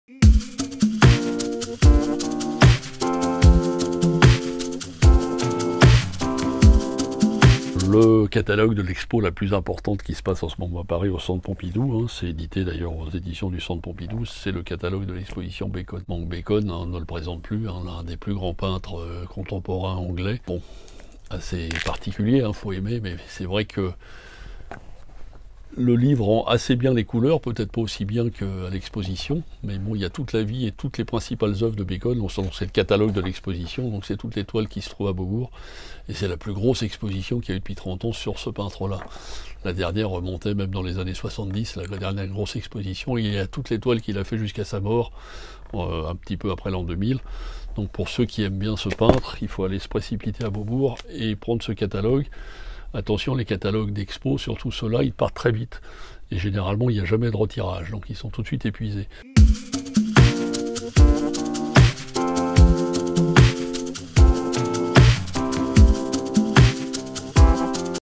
[LIVRE] LA CHRONIQUE DE JEAN-EDGAR CASEL - FRANCIS BACON EN TOUTES LETTRES
Francis Bacon en toutes lettres : Catalogue de l'exposition présentée au Centre Pompidou du 11 septembre 2019 au 20 janvier 2020 aux éditions Bibliothèque publique d'information du Centre Pompidou

La culture décontractée !

Merci pour votre soutien et votre amitié qui nous sont inestimables.
@Gérard Collard @Jean-Edgar Casel